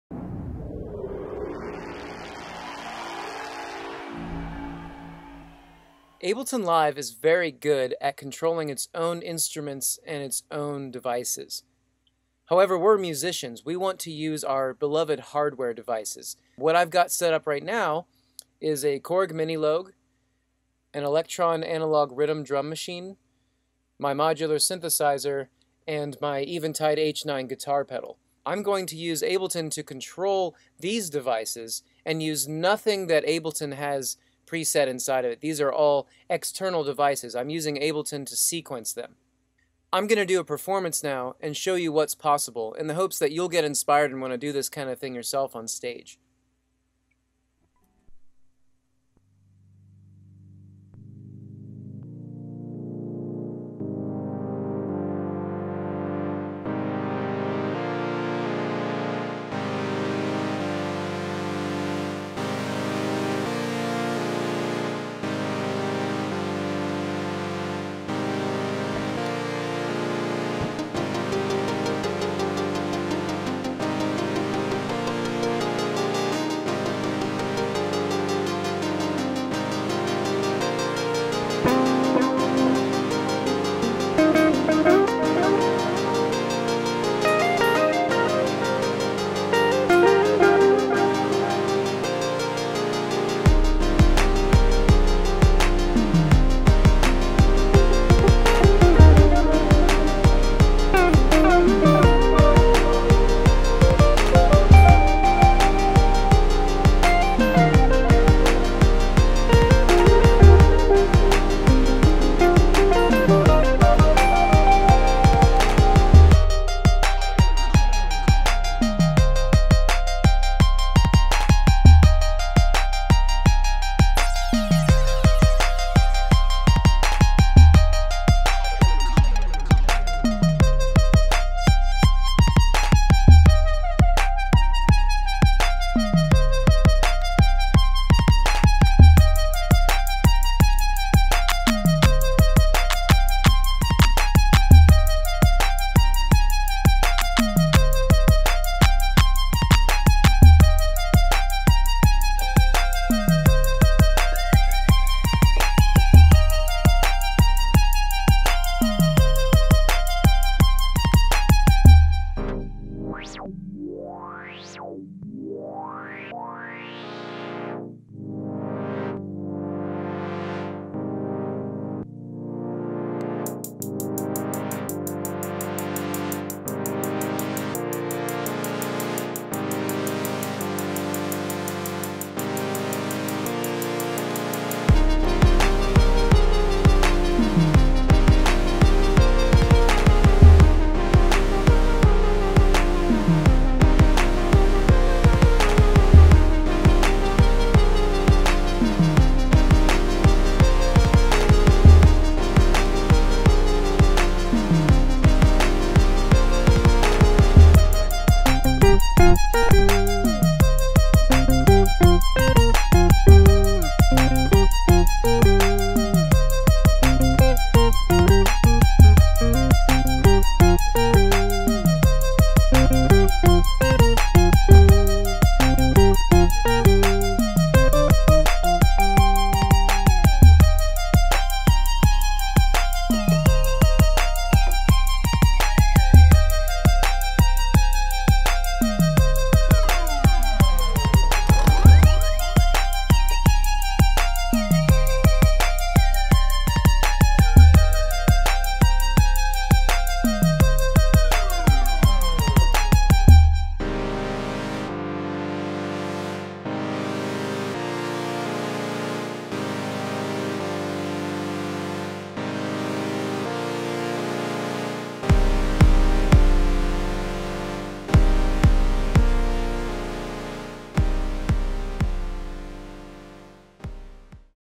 Using Ableton Live with Midi Hardware
Learn to use Ableton Live to sequence your midi keyboards, drum machines, effects, etc for stage and studio use!

Ableton Live is potentially the most robust and feature packed midi sequencer available. Many people dont realize this. You can control all aspects of midi CCs and program change simply using the available control parameters available in live clips! In this video I do a small performance with some midi gear I have in the studio to show what is possible. The sky is the limit! Please comment with questions and I will do my best to answer them.